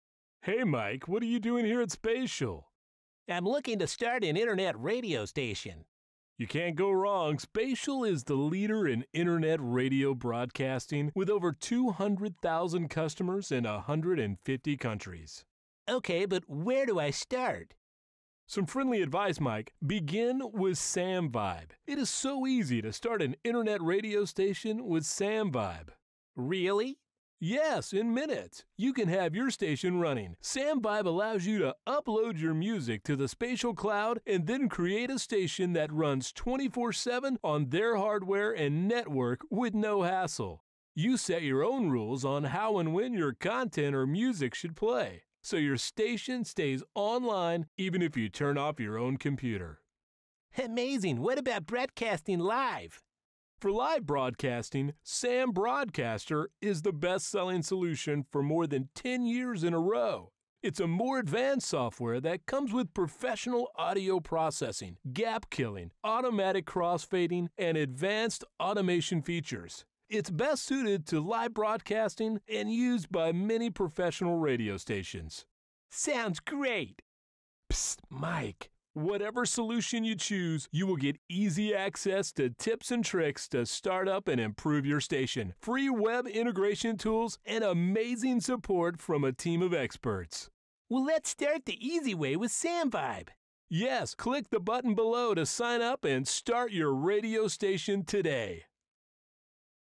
Introduction to Radio Broadcasting Solutions
What are the main differences between SAM VIBE and SAM Broadcaster?  Mike is having his Internet radio crash course today!